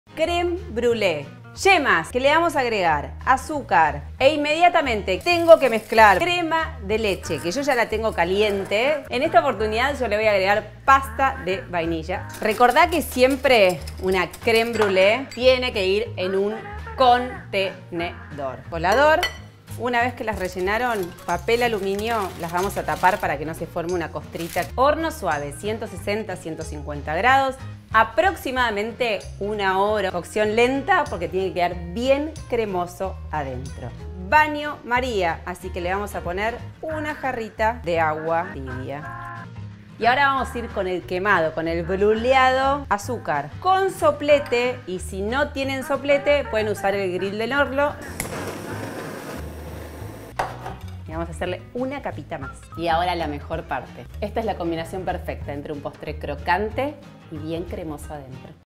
Creme Brûlée un postre deliciosa con el toque justo de humedad y crocante
Creme Brûlée el postre ideal con mucha humedad interior y un techito crocante que le da el toque especial.